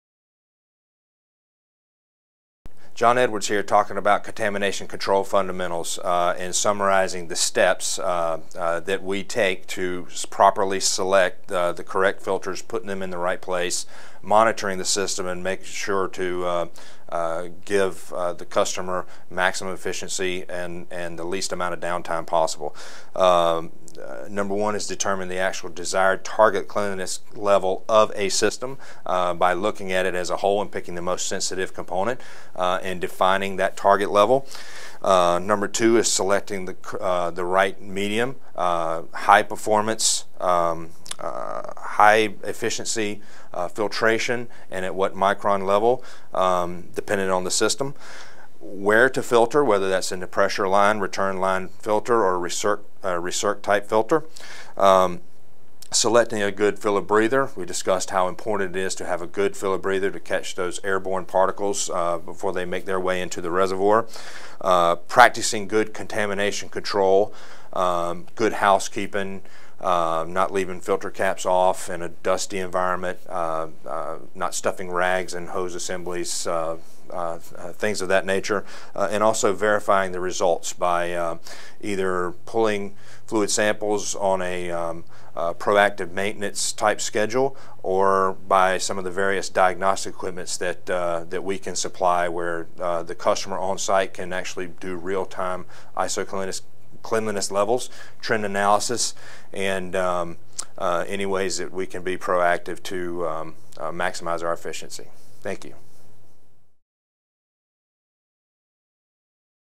How to Choose A HYDAC Filter
Transcript:
John Edwards here, talking about contamination control fundamentals and summarizing the steps that we take to properly select the correct filters, putting them in the right place, monitoring the system and making sure to give the customer maximum efficiency and and the least amount of downtime possible. Number one is determine the actual desired target cleanliness level of a system by looking at it as a whole and picking the most sensitive component and defining that target level. Number two is selecting the right medium, high-performance, high efficiency filtration and at what micron level, dependent on the system. Where to filter, whether that’s in the pressure line, return line filter or our research type filter. Selecting a good filler breather, we discussed how important is to have a good filler breather to catch those airborne particles before they make their way into the reservoir. Practicing good contamination control, good housekeeping, not leaving filter caps off in a dusty environment, not stuffing rags in hose assemblies. So, things of that nature and also, verifying the results by either pulling fluid samples on a proactive maintenance type schedule or by some of the various diagnostic equipments that we can supply where the customer on-site can actually do real-time iso-cleanliness levels, trend analysis and any ways we can be proactive to maximize our efficiency. Thank you.